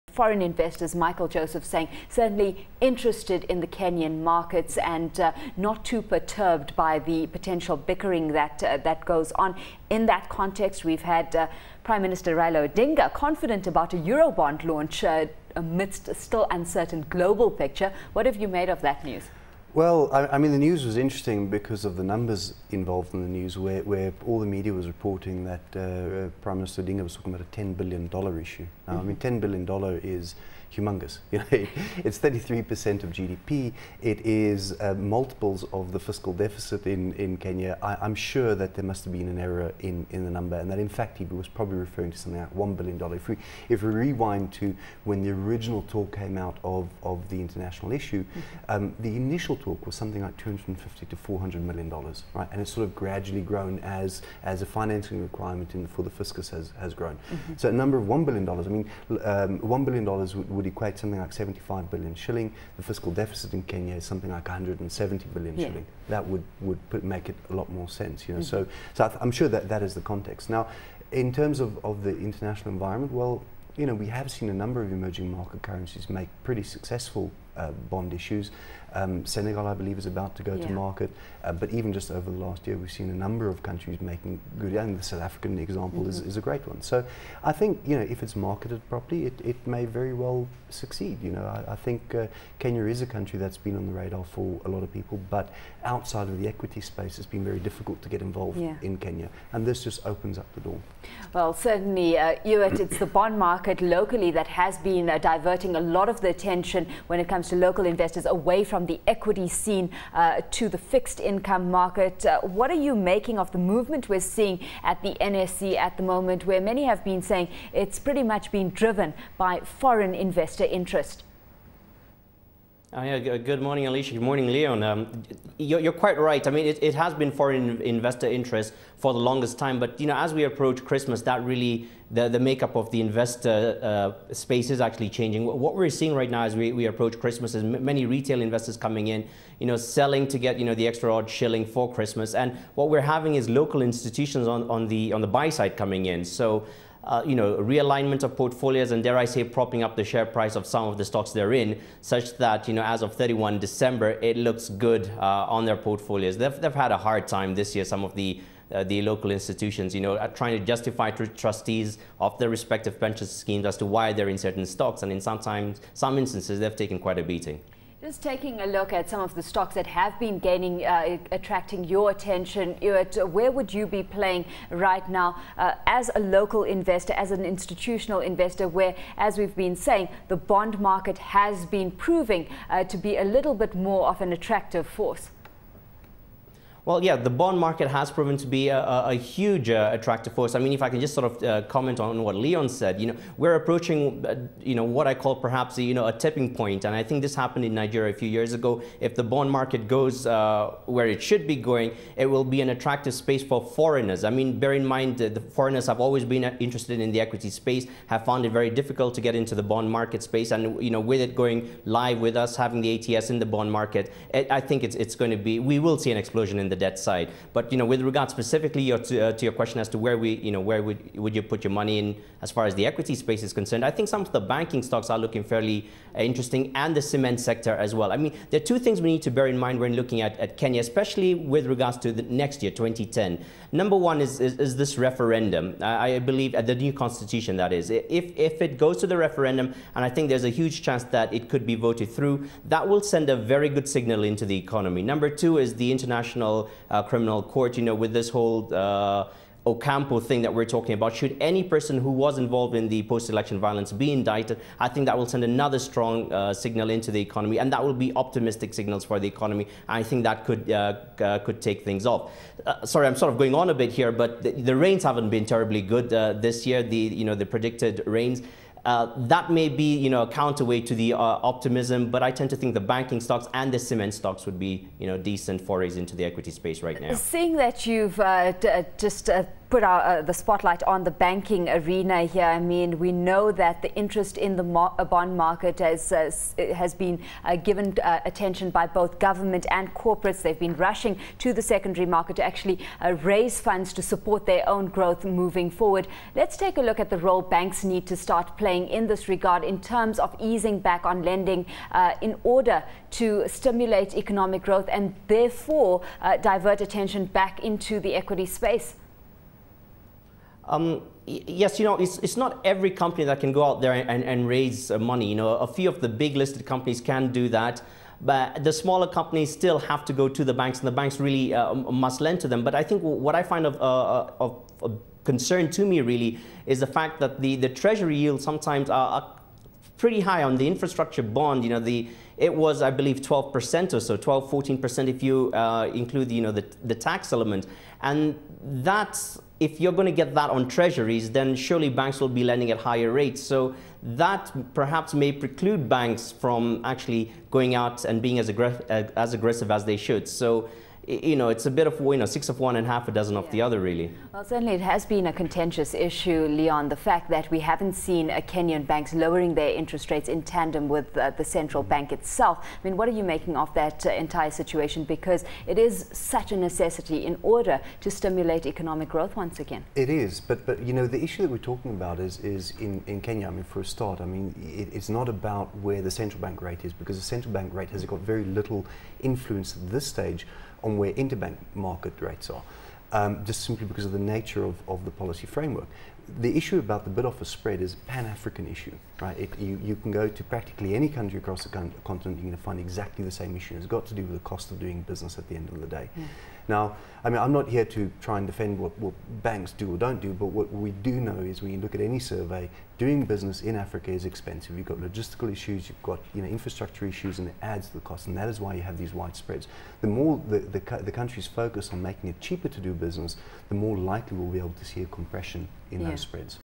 10 December - Kenyan Markets - Ewart Salins - African Alliance
Alishia Seckam speaks to Ewart Salins from African Alliance looking at:
Euro Bond launch
Movement on NSE
Bond Market